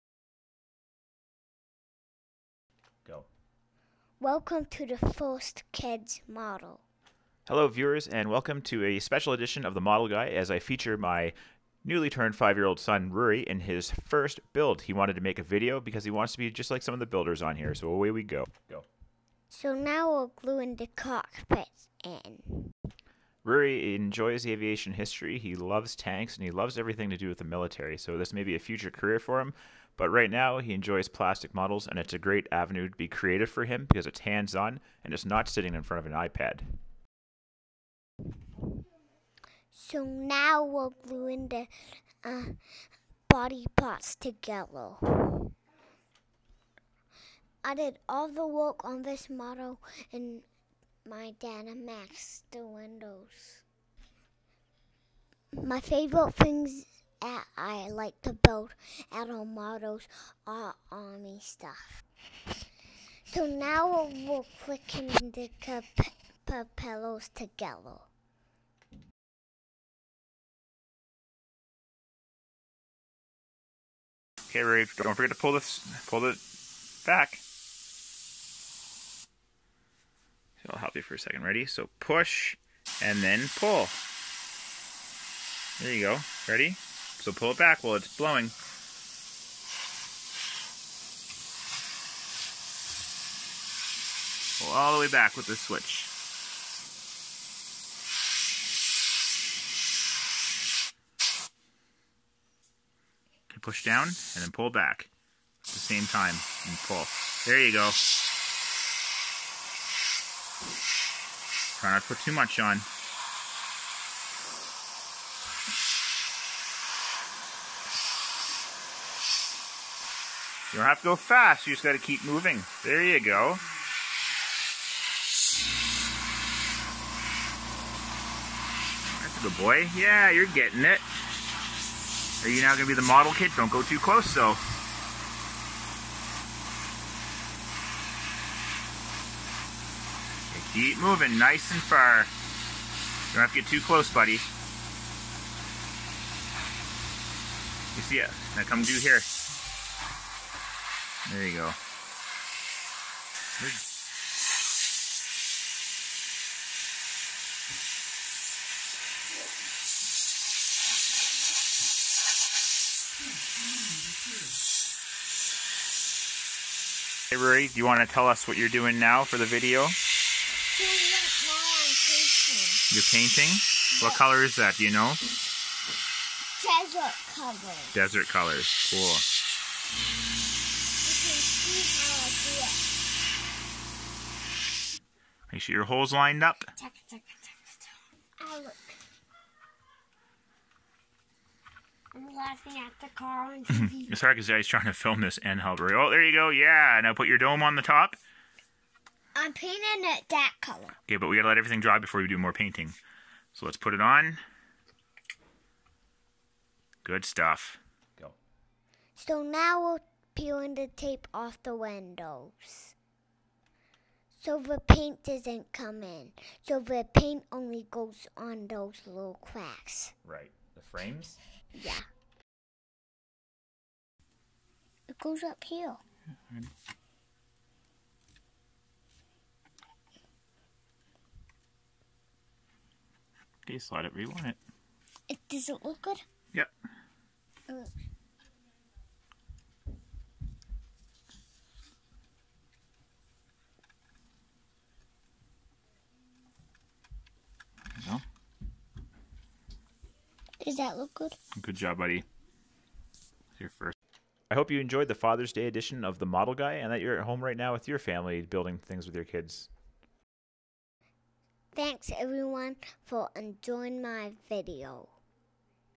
Happy Fathers Day!
I'm very excited to release a quick build video of my son's first model. He's done a quick snap-tite 1/72 Sea Stallion with only a few pointers from me. Ruairi wanted to do a video so he can be a Model Guy too like all the builders he's watched on YouTube. That is where my channel name comes from!

Get your kids engaged! Let them make messes and learn those hands-on skills that will help them later in life :)

Enjoy the special day to all the Fathers out there, especially the ones who can't be at home today.